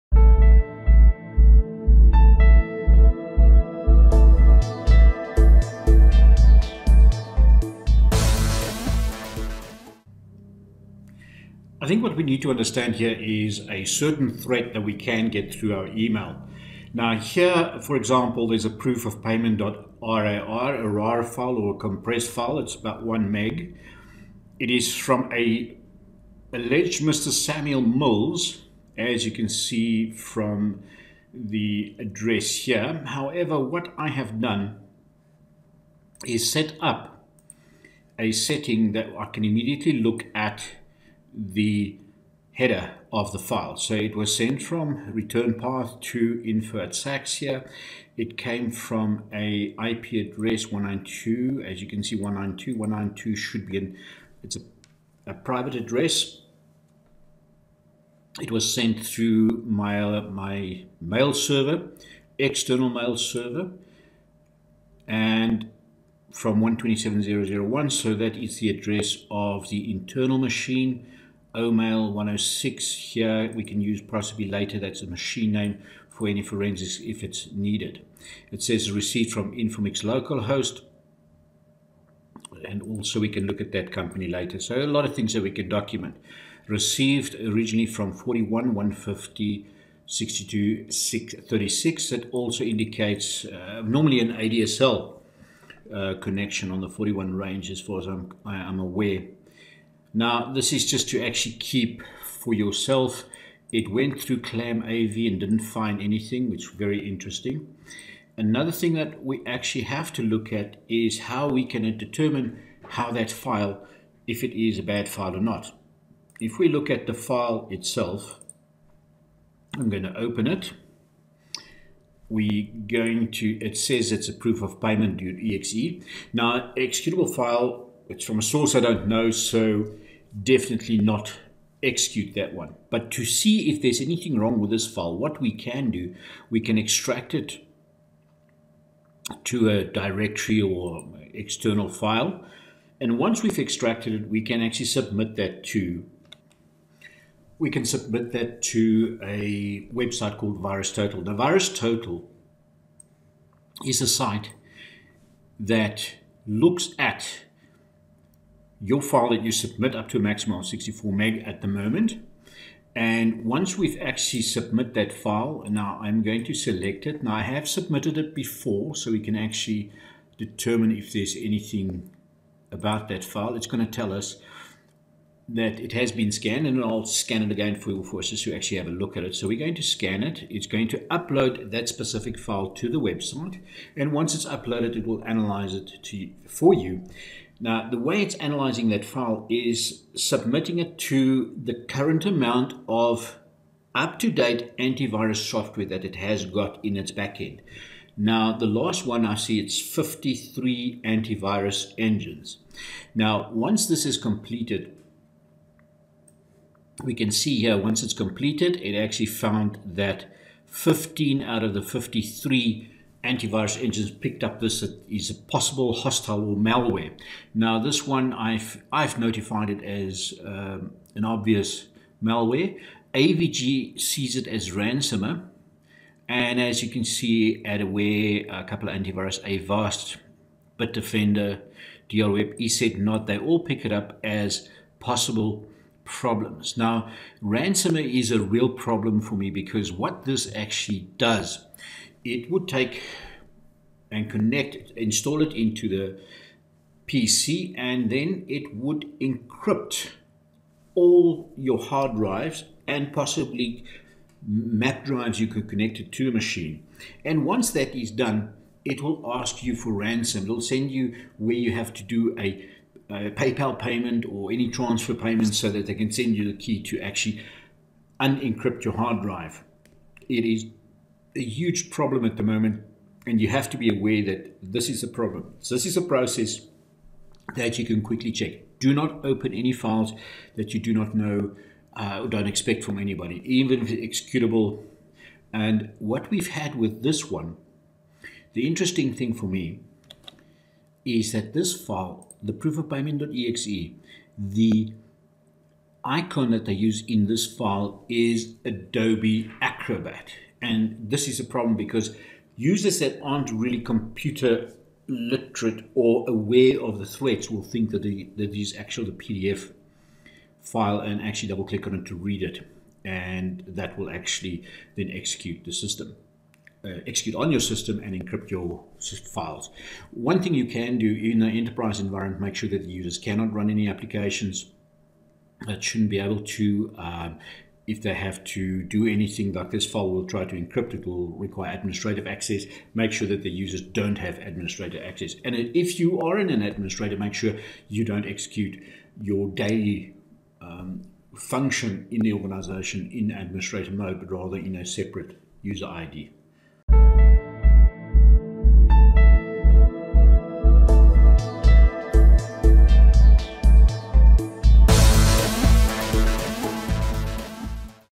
ransom
Ransomware is a current threat and users need to take cognisance of this high risk malware